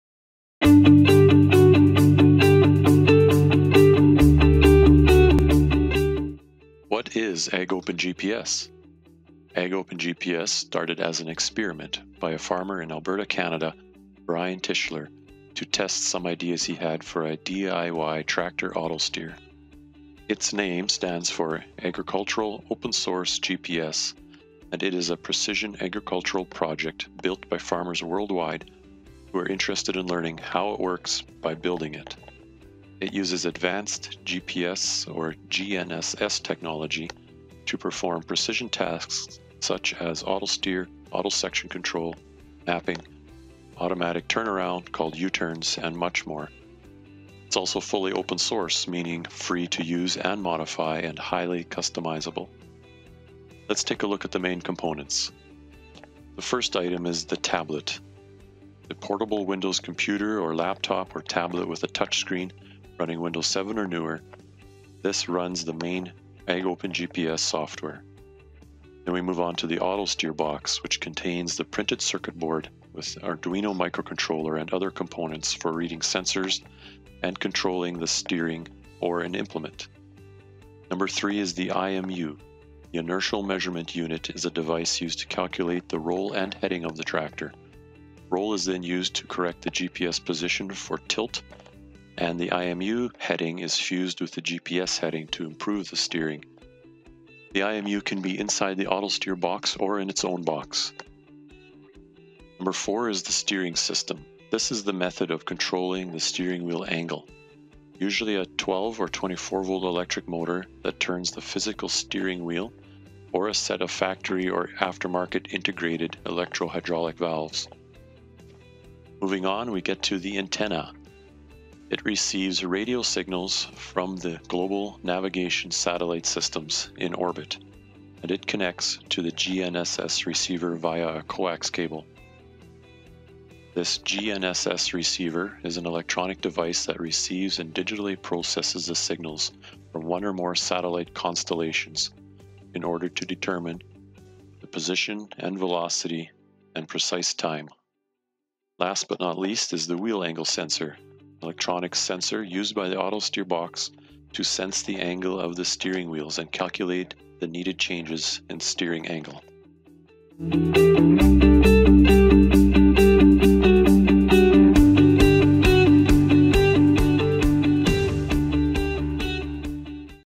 What is AgOpenGPS
AgOpenGPS started as an experiment by a farmer in Alberta Canada, Brian Tischler, to test some ideas he had for a DIY tractor autosteer.

It's name stands for Agricultural Open source GPS and it is a precision agriculture project built by farmers worldwide who are interested in learning how it works by building it. It uses advanced GPS/GNSS technology to perform precision tasks such as auto-steer, auto-section control & mapping, automatic turn around (called U-turns) and much more. It's also fully open source (meaning free to use & modify) and highly customizable.